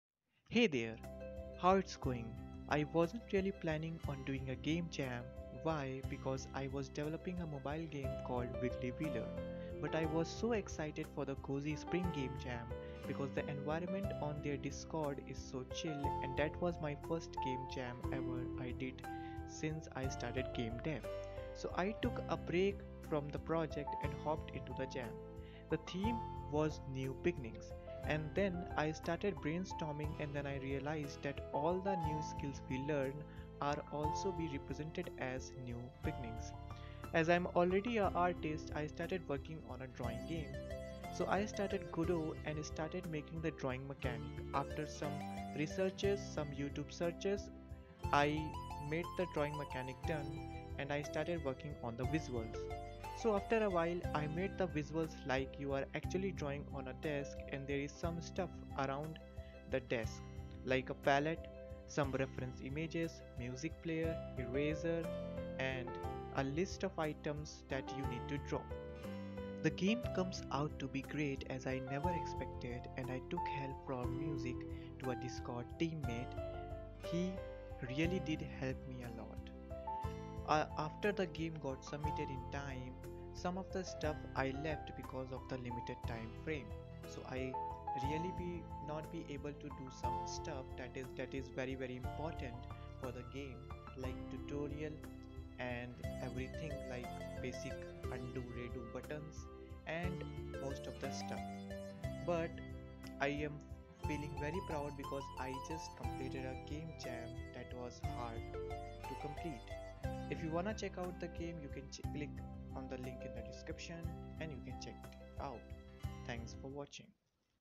I made a drawing game in Godot in 3 days !!
In this video I am going to tell you about How i participated in the game jam where I made a drawing game in godot engine!! Lets see how its turnout !!!